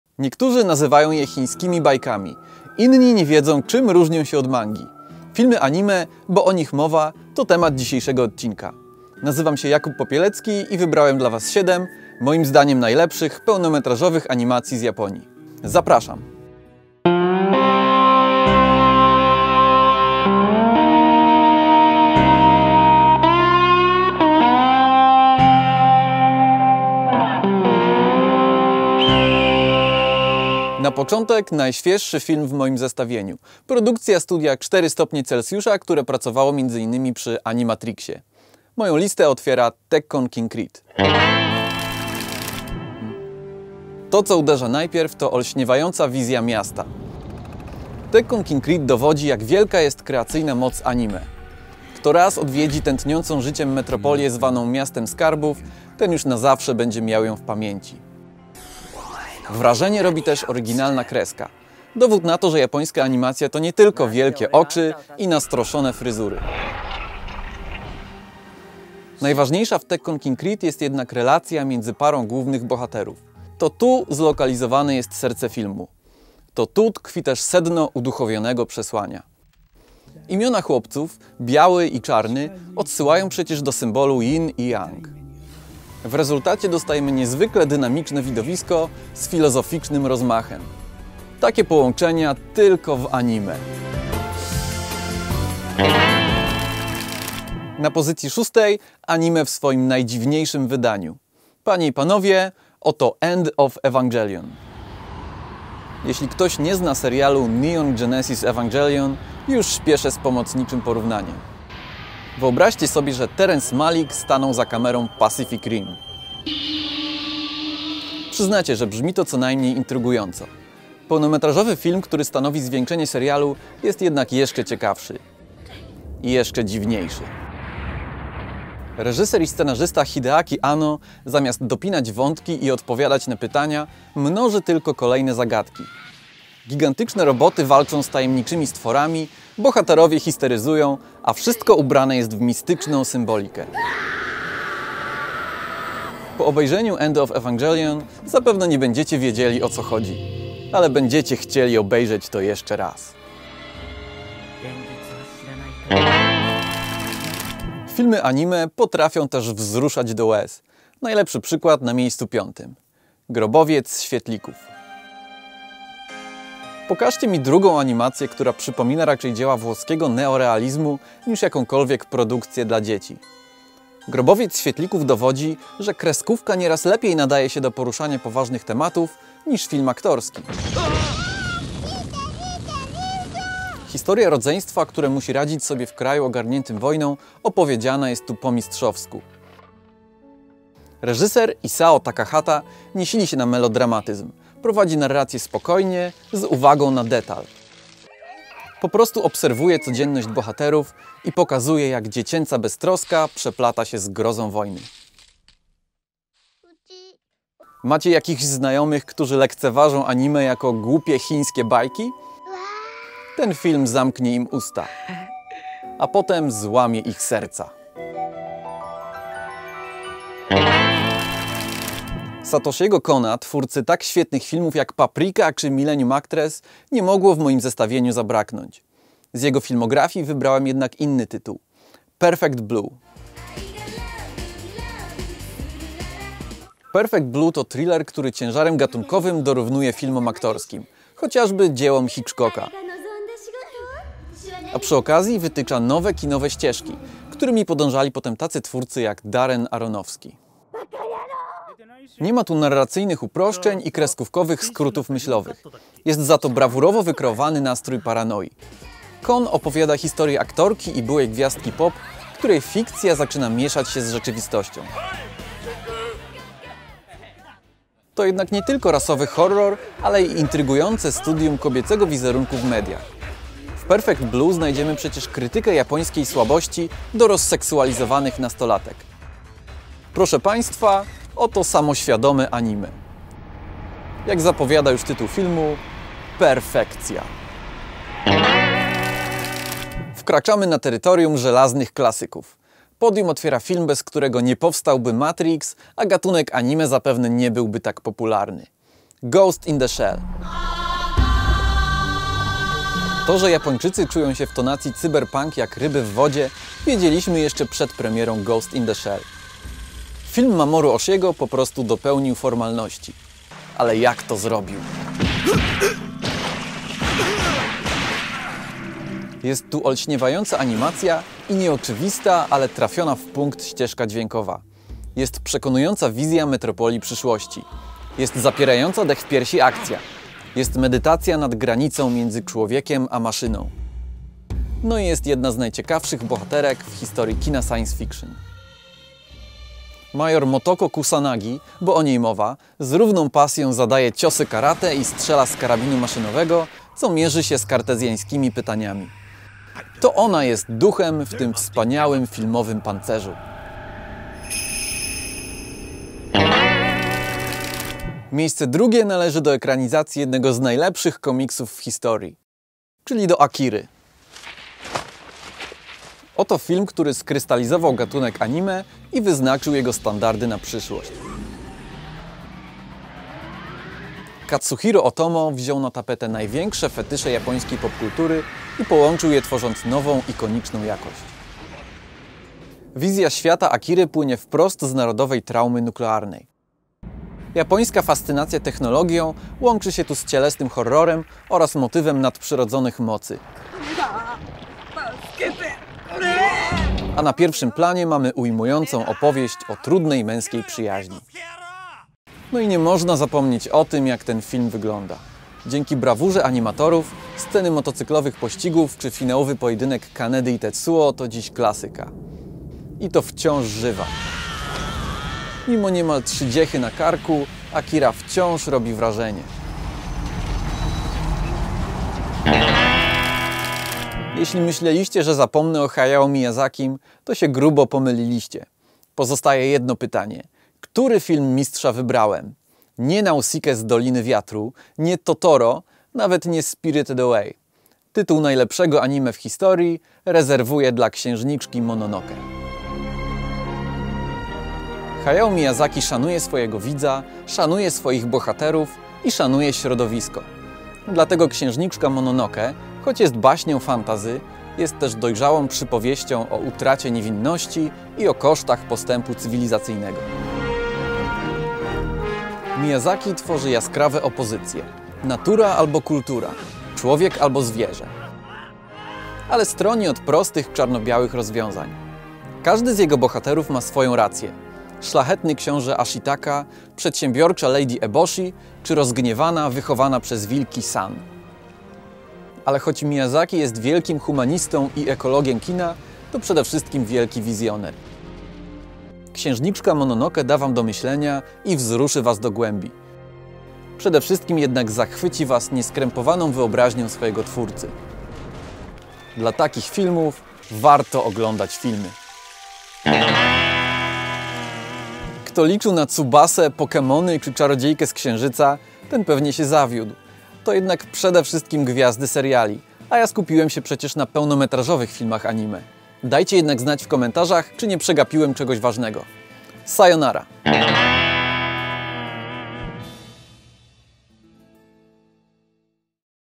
Anime to życie. Nie wierzycie? | FILMWEB
Japońskie filmy animowane stanowią bogatą tradycję, która obfituje w szereg rewelacyjnych produkcji. Szukamy tych najbardziej pamiętnych, przyglądając się filmografiom takich twórców, jak m.in. Katsuhiro Otomo, Hayao Miyazaki i Mamoru Oshii. Na którym miejscu znalazły się takie klasyki jak m.in. "Grobowiec świetlików", "Akira" i "Ghost in the Shell"? Zaprasza Jakub Popielecki.

Zobaczcie nasz wybór najlepszych filmów anime i koniecznie dajcie znać jakie są Wasze propozycje!

0:00 Anime - kraina wyobraźni
0:30 Cuda w mieście cudów
1:37 Neonowa ewangelia
2:37 Wojna nigdy się nie zmienia
3:42 Perfekcyjna niedoskonałość
4:49 Bez serc, bez ducha
5:58 Atomowa przyszłość
7:04 Eko-anime